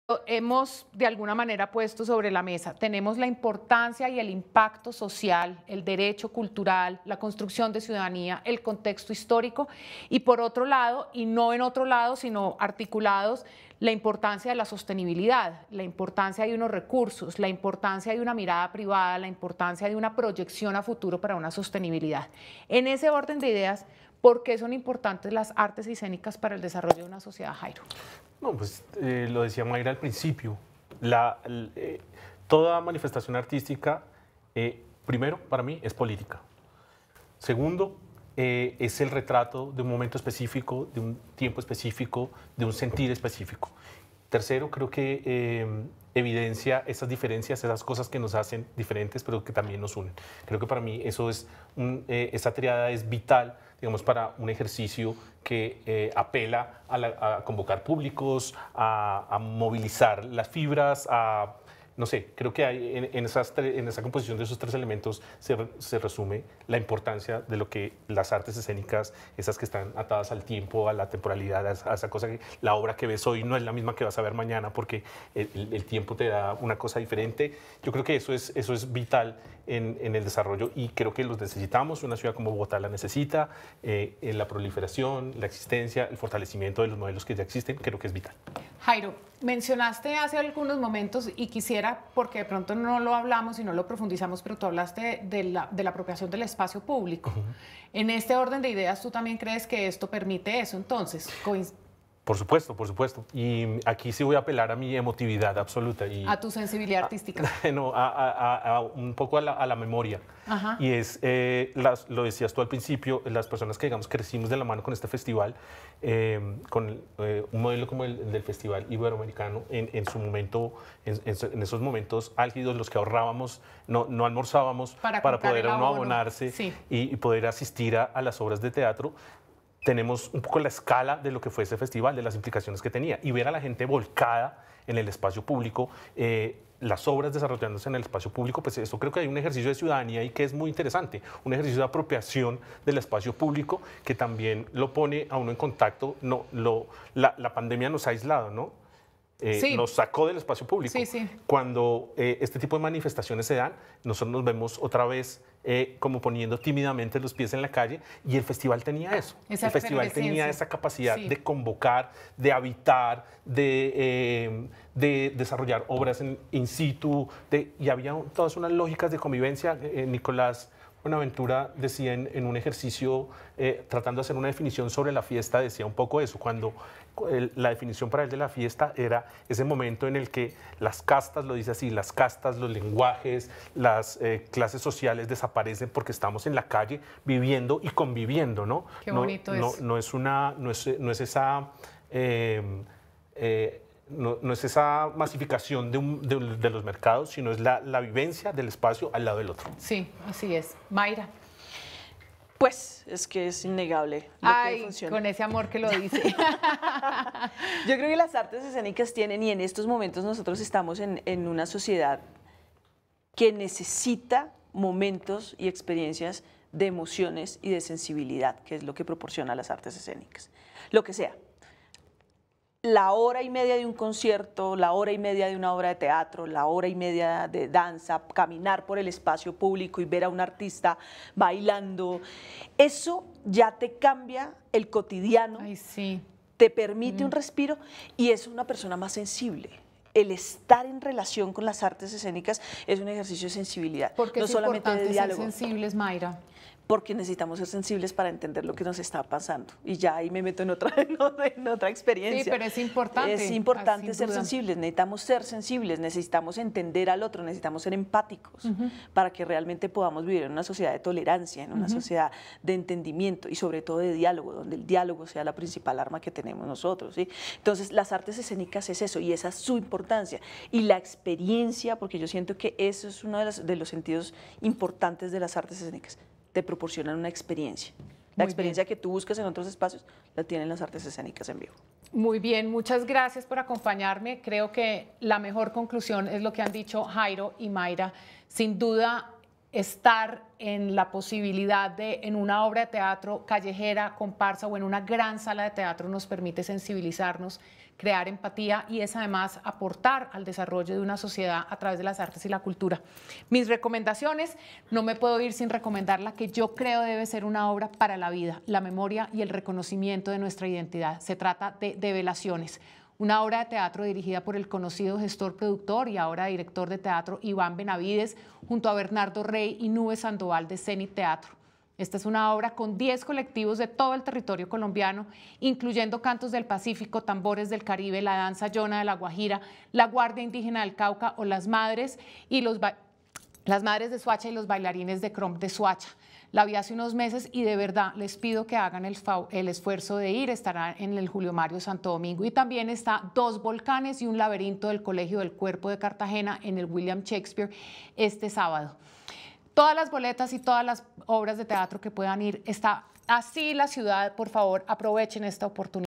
Las artes escénicas son un ejercicio de sensibilidad | Debate la cultura - Mesa Capital
MesaCapital | Para Jairo Suarez, director de mercados culturales de la Cámara de Comercio de Bogotá, las artes escénicas son políticas y motivan una apropiación del espacio en el que se realicen. Además te dejamos los recomendados de la semana de Catalina Ceballos.